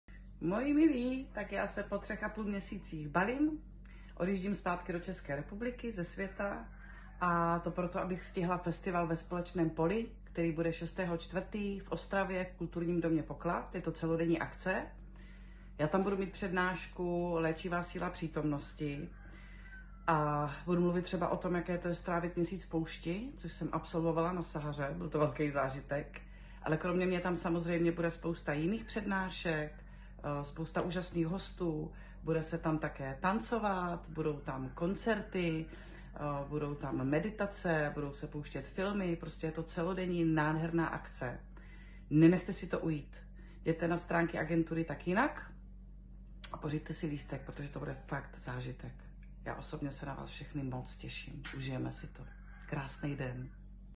Pozvánka od Sandry Pogodové na festival Ve společném poli
Festival Ve společném poli
6. dubna 2024 / DK Poklad OSTRAVA
SPIRITUALITA - ZDRAVÍ - SVOBODNÉ BYTÍ
ÚČINKUJÍCÍ
Léčivé divadlo Gabriely Filippi - TERAPIE RODINOU
Petr Casanova - Sandra Pogodová - Rostislav Václavek - Vendula Šmídke - Igor Chaun - Antonín Baudyš - MUDr. Monika Waksmundská - Jiří Charvát